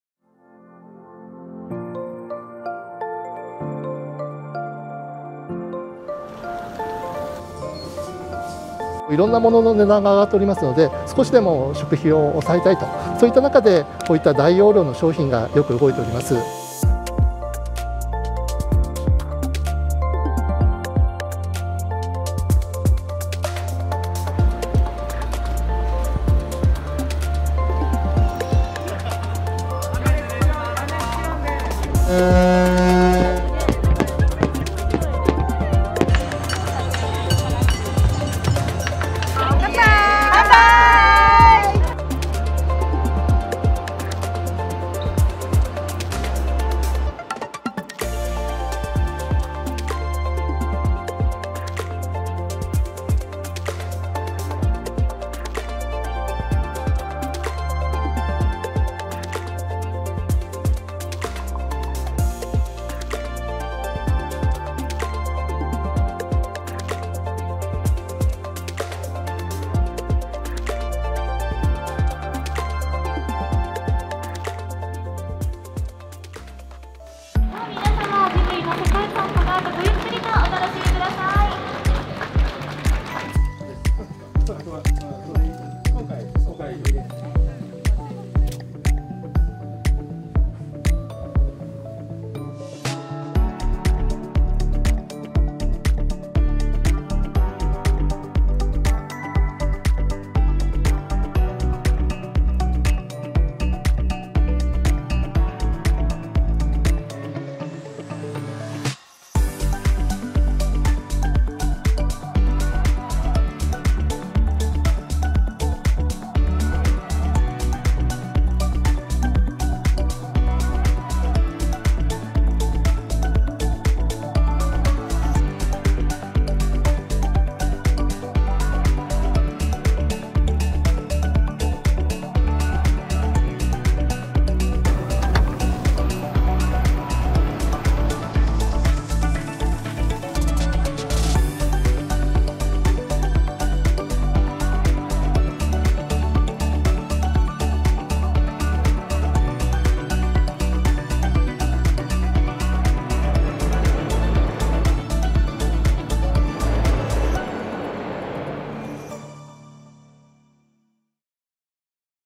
【22年ヒット商品番付】横綱は「コスパ＆タイパ」「3年ぶり」
日本経済新聞社は2022年の日経MJヒット商品番付をまとめました。東の横綱は「コスパ&タイパ」。あらゆる商品の値上げが続く中で、費用や時間に対する効果がより重視されるようになりました。西の横綱は新型コロナウイルス禍で中止されてきたイベントやレジャーの復活を示す「3年ぶり」を選びました。